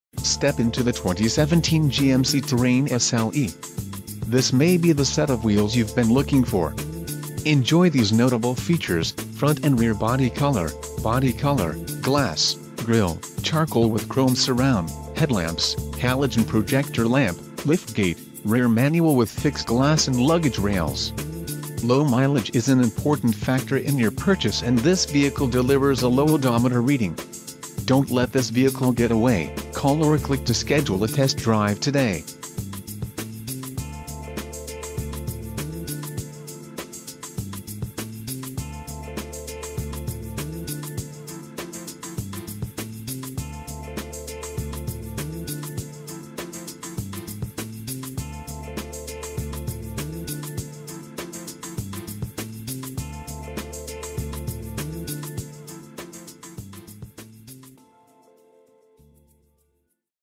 2017 GMC Terrain SLE - Kelly Car - Emmaus, PA 18049
2017 GMC Terrain SLE Condition: New Mileage: 6 Transmission: Automatic Exterior Color: White Doors: 4 Stock: G79015 Cylinders: 4